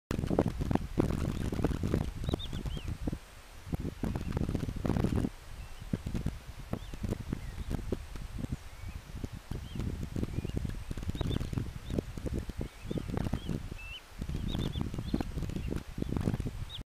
Flamencos, garzas y limícolas en Sueca (1 de 2)
Tres flamencos inmaduros de distintas edades aterrizaron en este campo de arroz desecado de Sueca el pasado día 3 de septiembre, y a pesar de la poca agua que quedaba, aguantaron hasta el día siguiente alimentándose en los charcos. Junto a ellos, limícolas de distintas especies aprovechan el último recurso alimentario, entre ellos correlimos menudos, comunes y zarapitines; archibebes comunes, oscuros y claros; andarríos bastardos, chicos y grandes; cigüeñuelas; chorlitejos grandes y chicos; agachadizas... en pocas semanas todos se habrán marchado y no los volveremos a ver hasta el paso prenupcial del año que viene, si bien algunas especies llegan a hibernar aquí.
Junto a ellos, descansan las garzas reales y las garcetas comunes, así como fumareles y pagazas.